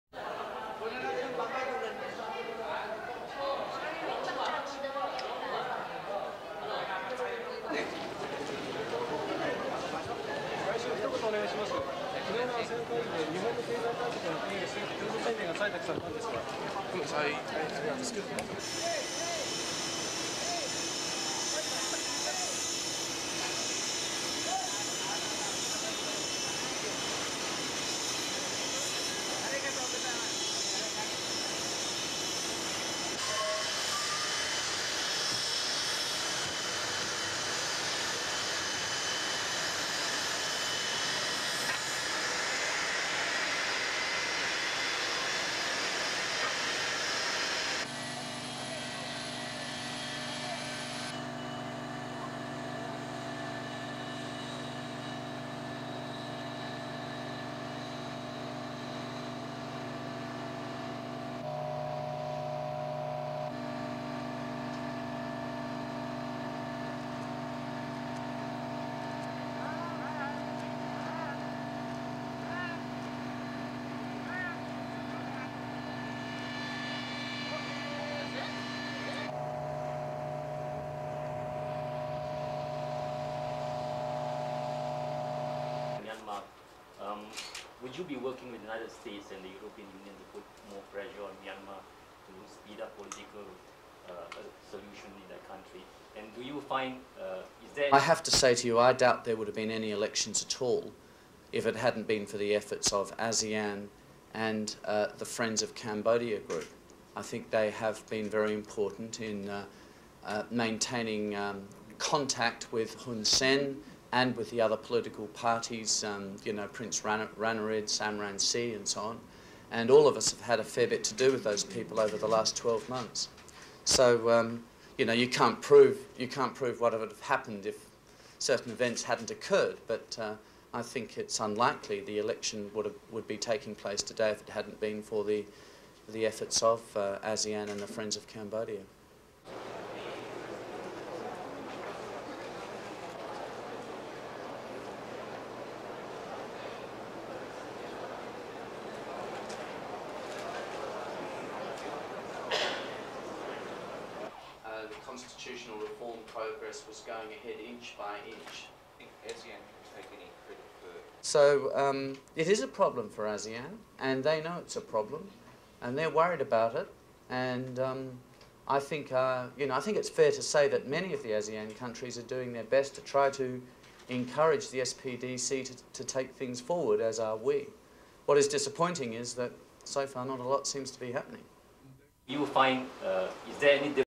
PHILIPPINES: KEIZO OBUCHI ATTENDS ASEAN CONFERENCE
The man expected to be Japan's next Prime Minister has arrived in Manila for a meeting of ASEAN leaders.

Keizo Obuchi will meet U-S Secretary of State Madeleine Albright on Sunday.

The ASEAN Conference is expected to be dominated by the elections in Cambodia which are also being held on Sunday.

Keizo Obuchi is making his debut as prime minister designate of Japan, during a brief trip to Manila for the ASEAN conference.

Obuchi has just been elected president of the ruling Liberal Democrat Party (L-D-P) and will almost certainly be elected as Japan's new prime minister at a special parliamentary sitting on Thursday.

After arriving in Manila, the premier-in-waiting is expected to hold separate talks with U-S Secretary of State Madeleine Albright.

Albright and Obuchi had not previously been scheduled to meet separately, but agreed to a quick dialogue session on Sunday, a few hours after their arrivals.

He is also expected to meet the Russian Foreign Minister Yevgeny Primakov and Chinese Foreign Minister Tang Jiaxuan.

Obuchi will later attend an informal dinner with foreign ministers, where he is expected to unveil his ideas on political, economic and diplomatic policies.

High on the agenda at the conference will be the parliamentary elections being held in Cambodia on Sunday.

Cambodia is scheduled to become the tenth member of ASEAN next year, but other member countries have said that free and fair conduct of the elections are a precondition for entry.

Australian Foreign Minister Alexander Downer claimed ASEAN had been pivotal in bringing about the election in Cambodia.

SOUNDBITE: (English)
"I have to say to you that I doubt there would have been any elections at all, if it hadn't been for the efforts of ASEAN and the Friends of Cambodia Group. I think they have been very important in maintaining contact with Hun Sen, and with the other parties, you know, Prince Ranariddh, Sam Rainsy and so on. And all of us have had a fair bit to do with those people over the last twelve months, so you can't prove what would have happened if certain events hadn't occurred, but I think it's unlikely that the election would have taken place today if it hadn't been for the efforts of ASEAN and the Friends of Cambodia"
SUPER CAPTION: Alexander Downer, Australian Foreign Minister

Downer was more cautious in his appraisal of the prospects of democratisation in Myanmar.

SOUNDBITE: (English)
"So it is a problem for ASEAN and they know it's a problem, and they're worried about it and I think its fair to say that they are doing their best to encourage the SPDC (Myanmar Government) to take things forward as are we. What is disappointing is that so far not a lot seems to be happening"

ASEAN has been criticised for admitting Myanmar into the group last year, because of that country's poor record on human rights and lack of a democratic process.